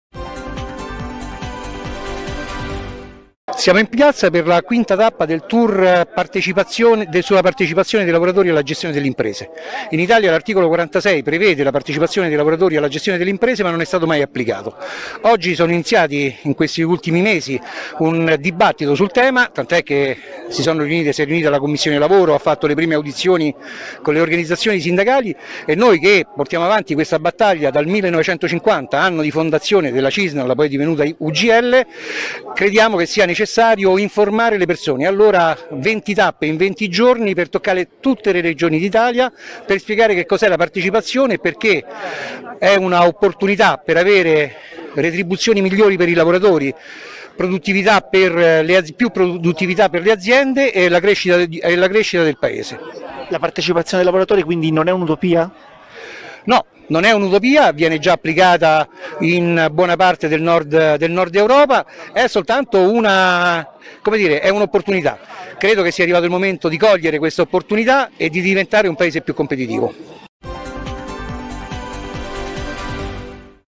Capone "Favorire partecipazione dei lavoratori a gestione imprese"
CATANIA (ITALPRESS) - "La partecipazione dei lavoratori alla gestione delle imprese è un'opportunità per avere retribuzioni migliori per tutti, più produttività per le aziende e crescita per il Paese. La partecipazione dei lavoratori, quindi, non è un'utopia; viene già applicata in buona parte del nord Europa. Credo che sia arrivato il momento di cogliere questa opportunità". Lo ha dichiarato il segretario generale dell'UGL, Paolo Capone, presente alla tappa catanese del tour LavoroèPartecipAzione organizzato in occasione del 74° anniversario della Confederazione. xn5/vbo